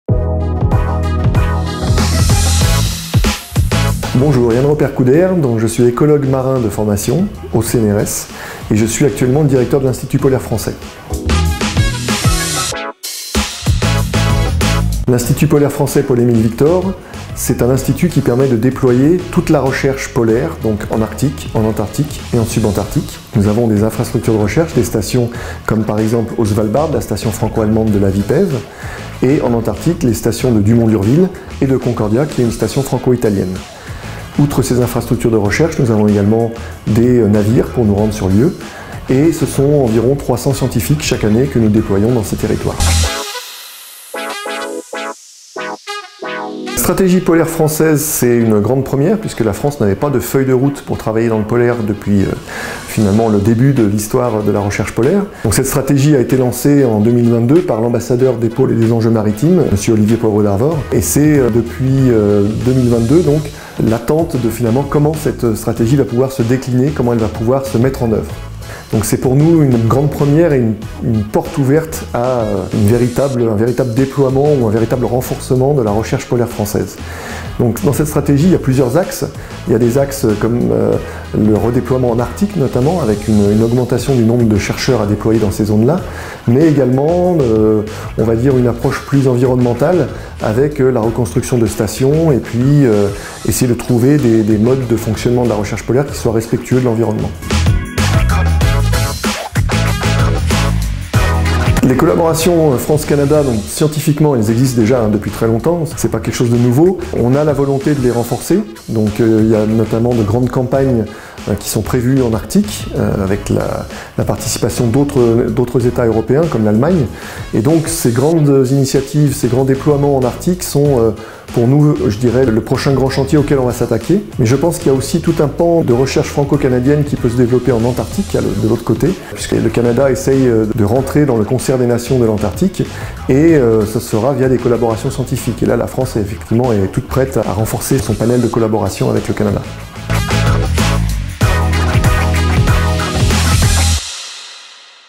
Entretien avec Yan Ropert-Coudert
Yan Ropert-Coudert 🇫🇷, écologue marin de formation, au CNRS - Centre national de la recherche scientifique, et actuel directeur de l'Institut polaire français Paul-Émile Victor, nous parle du renforcement de la recherche polaire française depuis 2022 et des collaborations France 🇫🇷 - 🇨🇦 Canada

[EN]

Yan Ropert-Coudert, 🇫🇷 marine ecologist at the 🇫🇷 CNRS - Centre national de la recherche scientifique and currently director of the French Polar Institute, talks to us about the strengthening of French polar research since 2022 and France 🇫🇷 - 🇨🇦 Canada collaborations